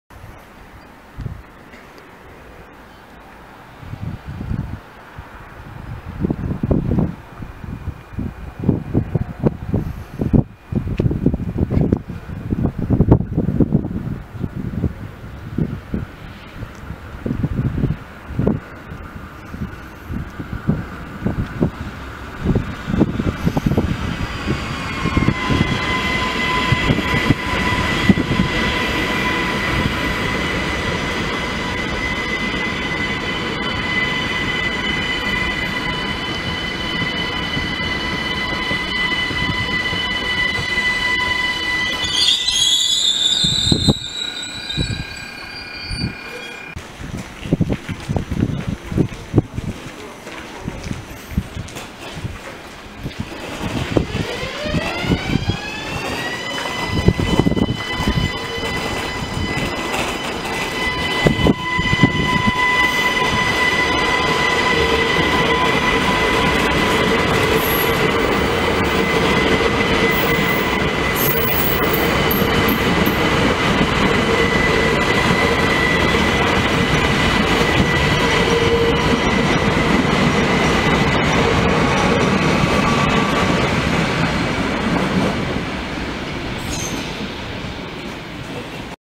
REX 2128 Cityjet Bf. Gmünd NÖ
Der REX 2128 Wien FjBf - České Velenice bei der Ein- und Ausfahrt Bf. Gmünd NÖ. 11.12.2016